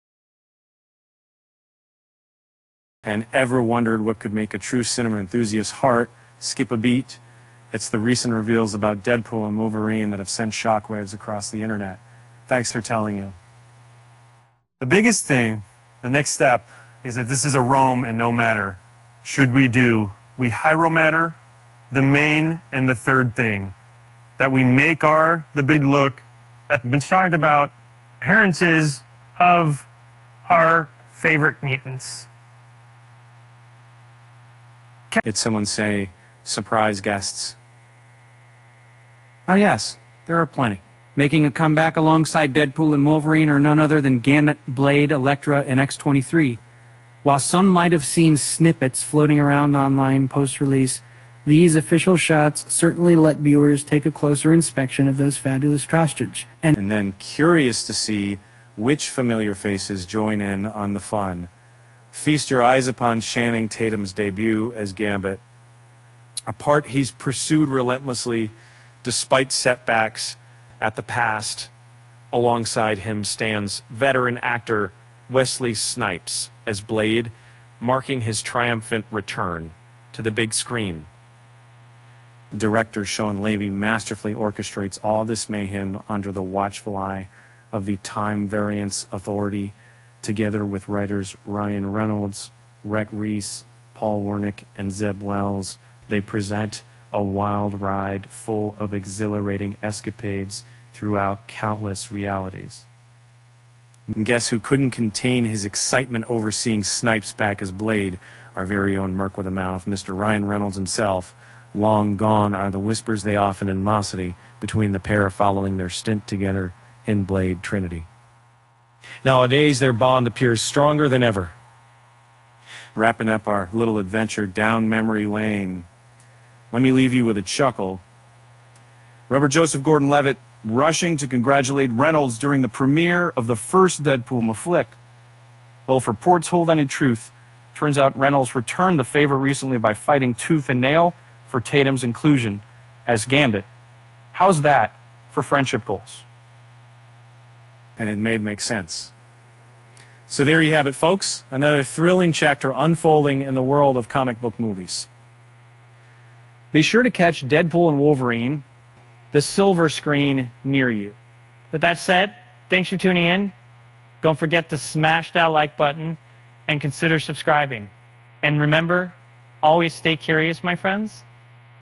Deadpool & Wolverine Shock Cameos Finally Officially Revealed by Shawn Levy in New Images
Ever wondered what could make a true cinema enthusiast's heart skip a beat? It's the recent reveals about Deadpool and Wolverine that have sent shockwaves across the internet! Thanks to the generosity of Director Shawn Levy, we've gotten our hands on some stunning behind-the-scenes photos from 'The Void'. These sneak peeks give us an up close and personal look at the much-talked-about appearances of our favorite mutants.